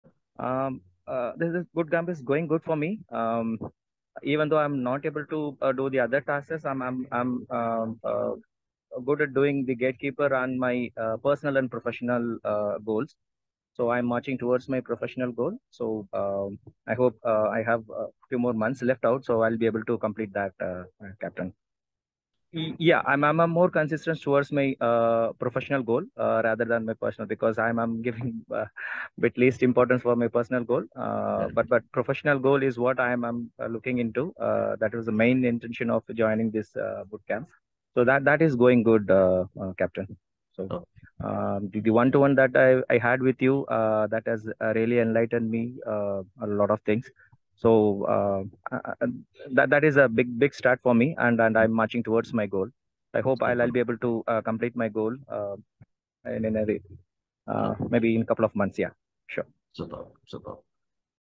Success Story | How Mani used the Art of Discipline principles to become a better stock trader
Mar 2024 Batch | Manigandan's Success Story | Will Boot Camp

Manigandan's Success Story | How she has remained consistent since 2020 using NZD technique

WhatsApp AOD or Art of Discipline to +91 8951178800